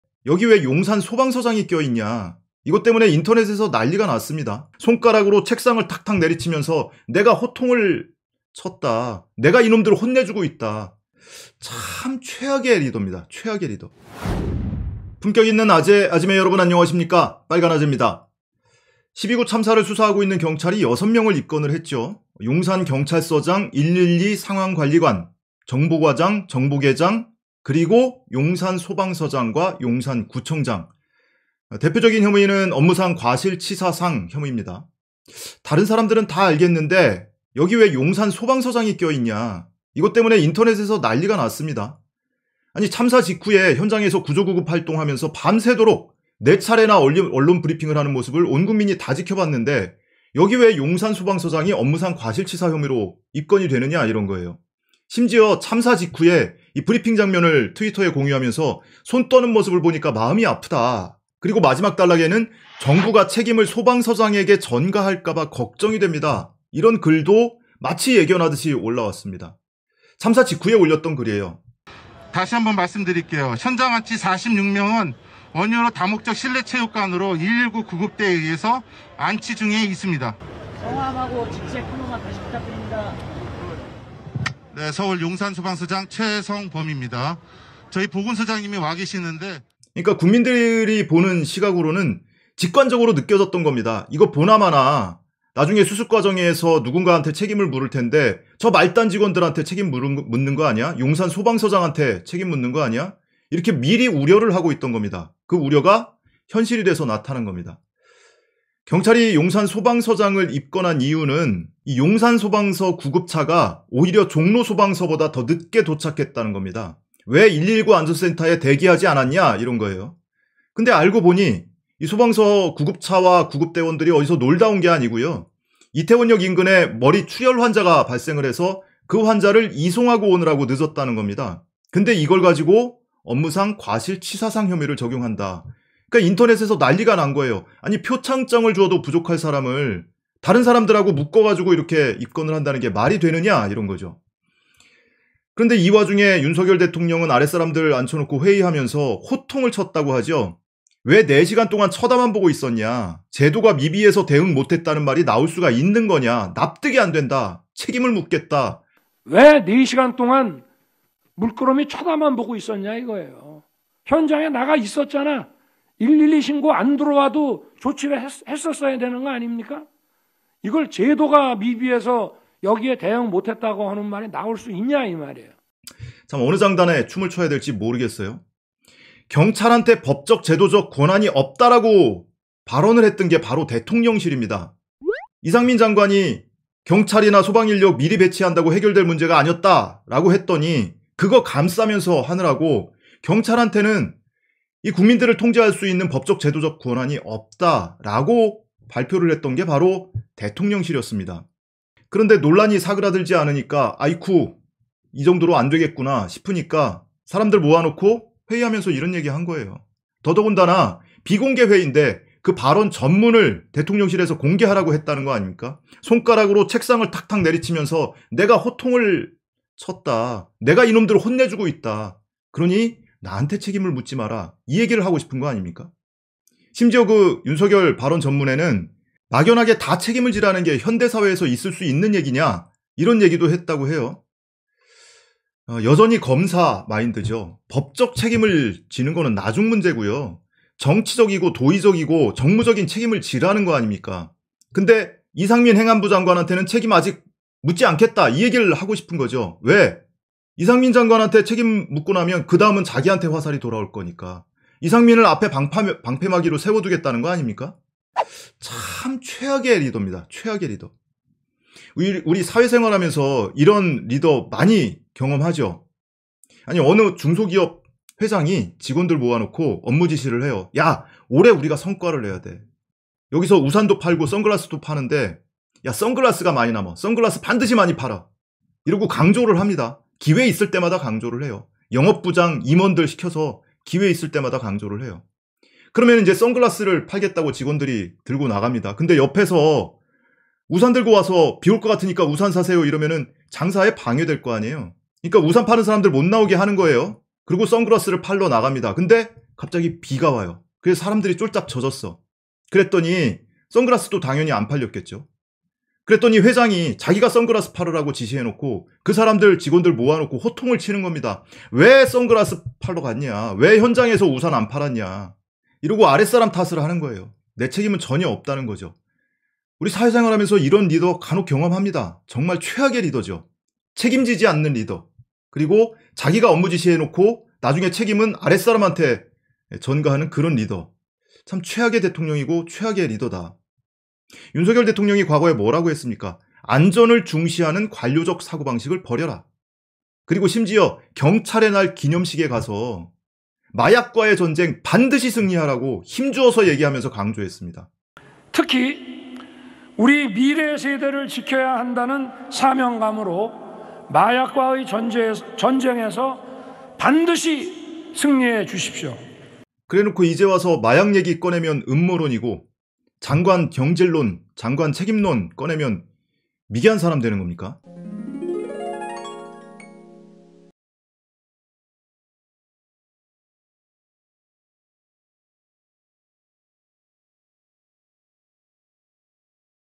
"소방서장에 책임 전가 걱정" 용산소방서장 입건 예언한 트위터리안 [빨간아재]
경찰청 특별수사본부가 용산소방서장을 입건한 사실이 알려지면서 시민들의 공분이 커지고 있습니다.

더구나 트위터에선 지난 30일 손을 떨며 브리핑하는 용산소방서장의 움짤을 공유하며
용산소방서장에게 책임을 전가할까봐 걱정된다는
우려 섞인 예언까지 있던 터여서 더욱 고개를 갸웃하는 시민이 많아지고 있습니다.

이런 가운데 윤석열 대통령은 "왜 4시간 동안 쳐다만 보고 있었냐. 제도가 미비해서 대응 못했다는 건 납득이 안 된다"며 스스로 대통령실의 브리핑을 뒤집었습니다.